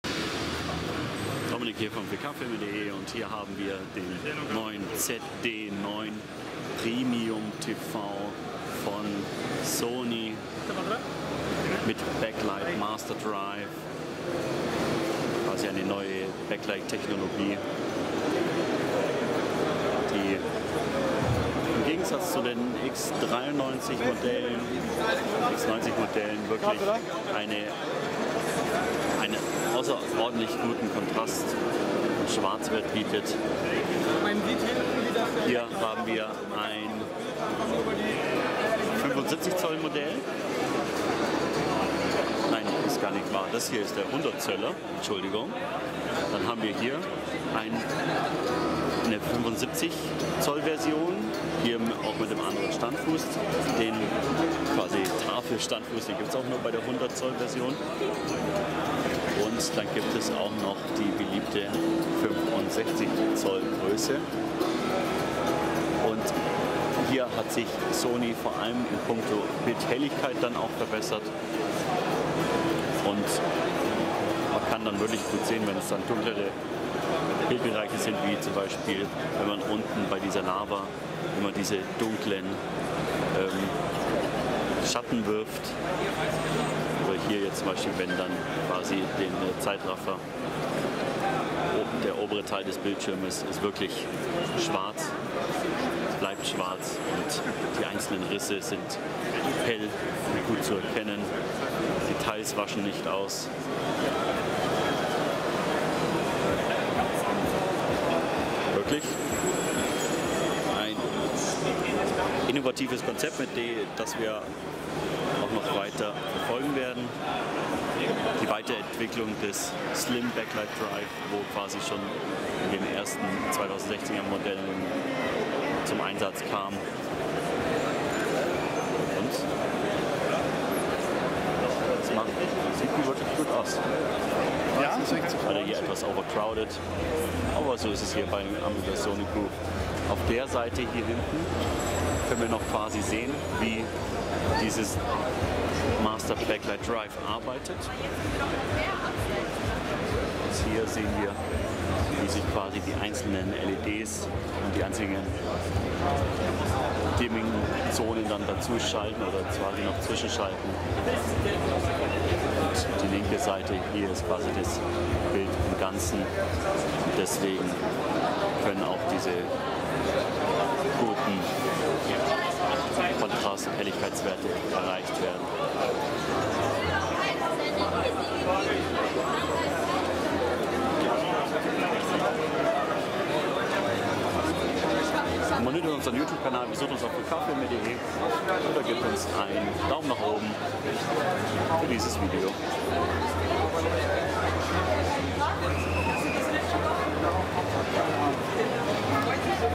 SONY ZD9 4K UHD Fernseher mit Backlight Master Drive @ IFA 2016
Sony zeigt seine neuen Top-Modelle der ZD9 Serie mit 100, 75 und 65 Zoll Diagonale. Die ZD9 Serie führt auch die neue Backlight-Master-Drive Technologie von Sony ein, die ungeahnte Farben, Kontraste und Schwarzwerte auf den Bildschirm zaubert. Die Funktionalität des Backlight Master Drive erklären wir ebenfalls im Video.